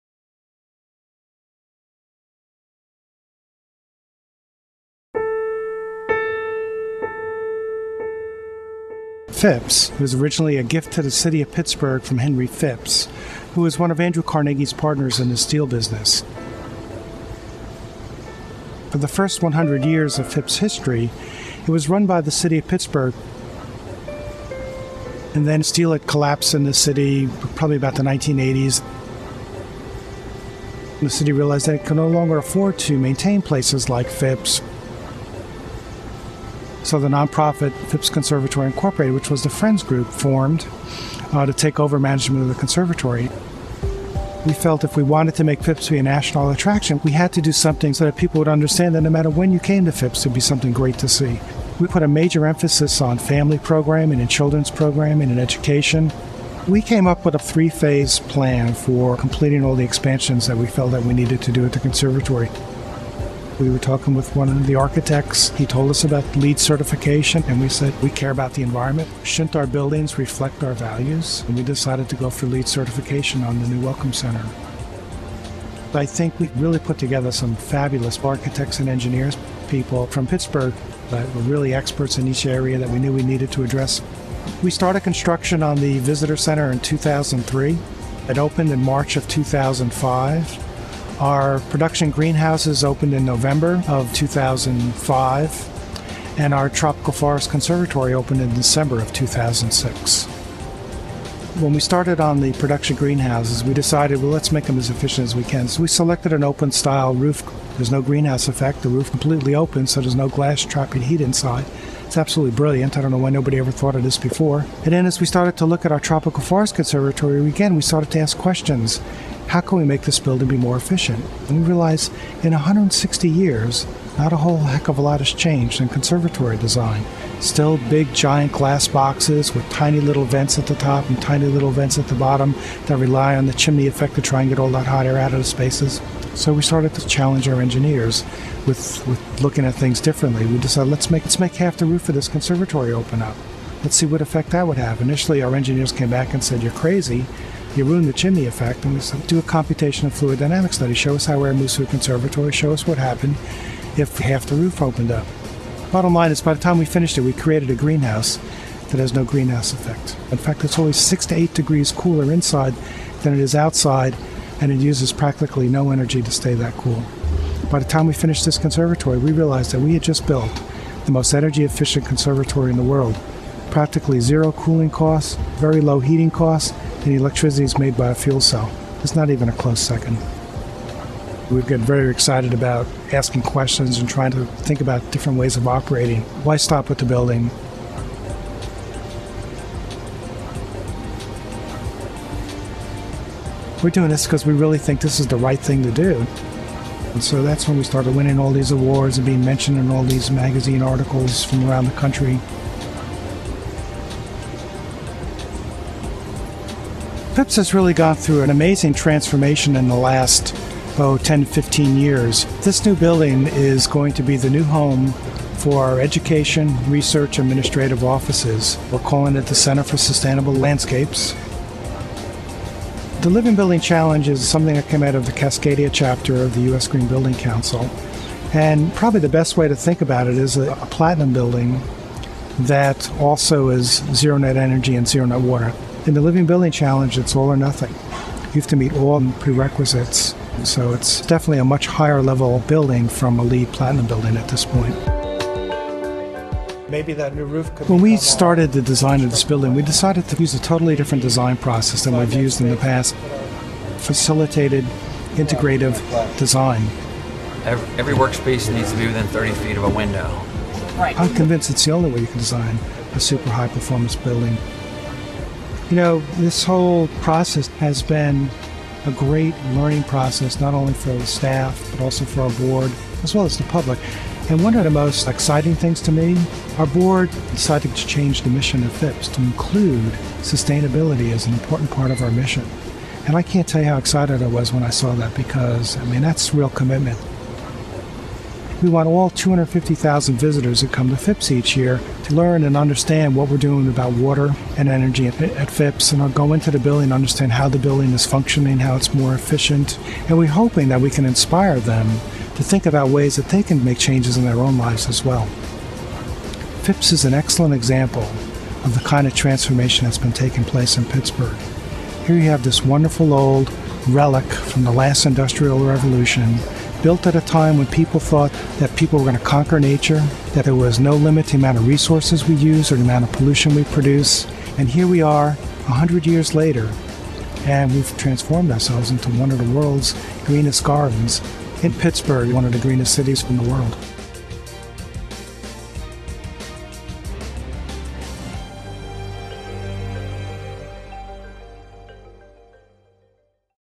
The Greening of Phipps: A Conversation with the Executive Director
Richard Piacentini, executive director of Phipps Conservatory and Botanical Gardens in Pittsburgh since 1994, talks about the history of the public garden's1893 Victorian glasshouse and how the organization has transformed into a premier green destination.